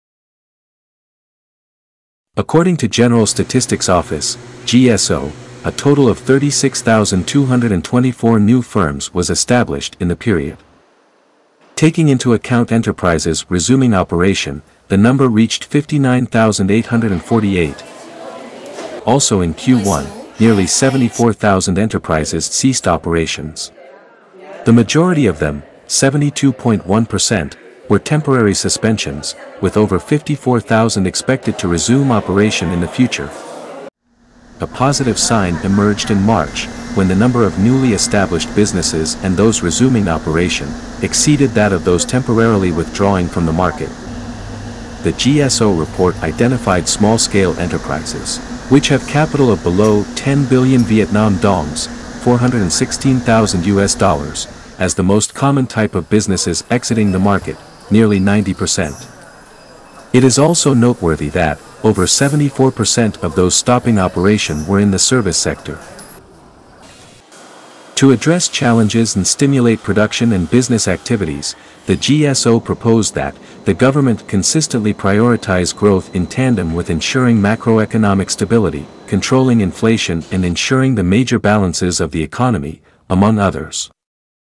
Number of new businesses hits decade high in Q1
The General Statistics Office (GSO) reported a decade-high number of new businesses established nationwide in the first quarter.